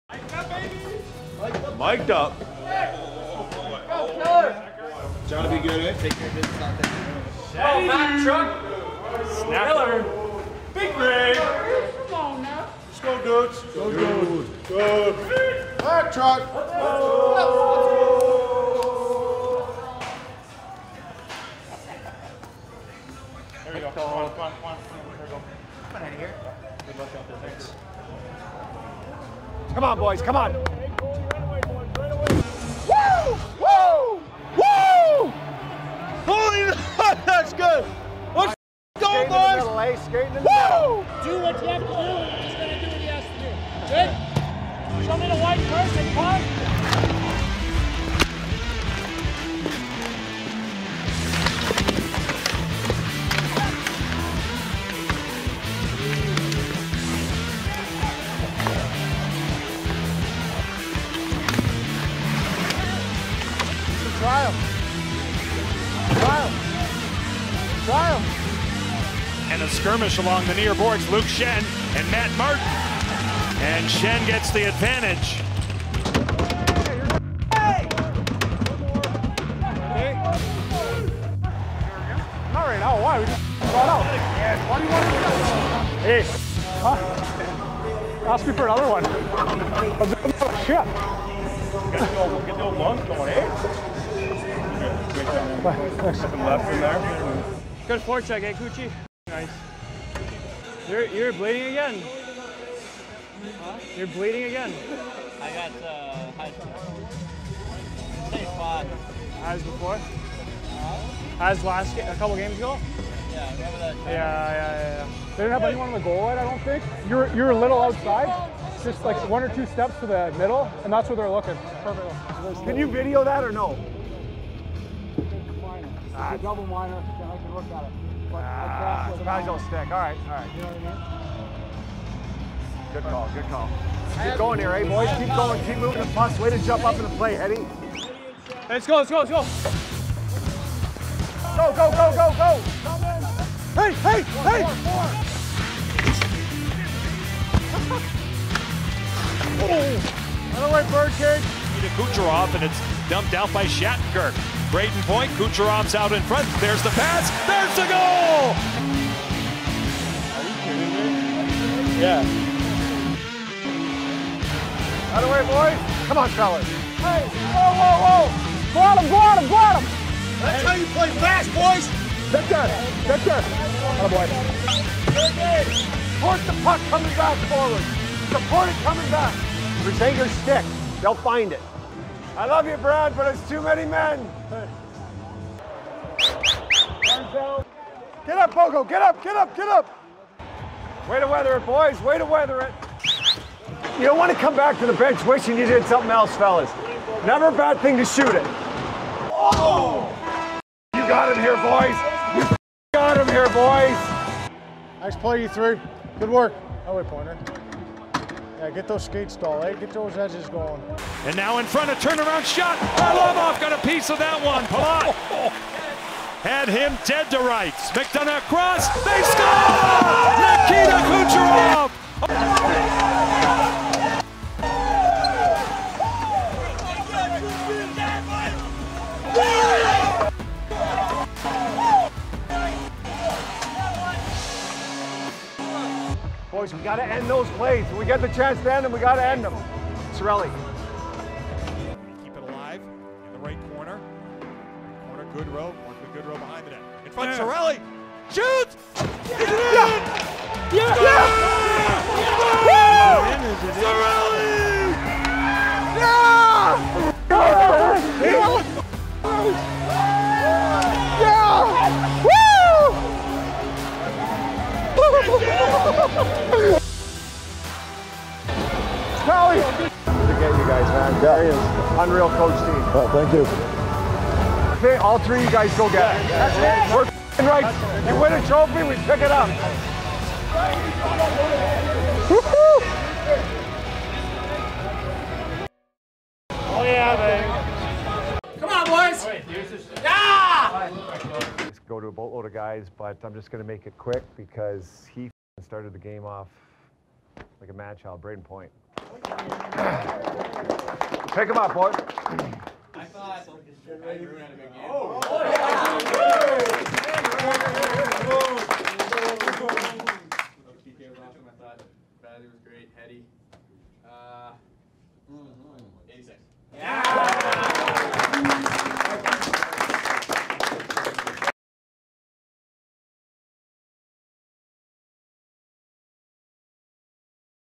Wired For Sound | Best of ECF vs New York Islanders
LISTEN IN as Anthony Cirelli, Jon Cooper, Luke Schenn, Barclay Goodrow, and Pat Maroon wore mics during their Eastern Conference Finals matchup versus the New York Islanders.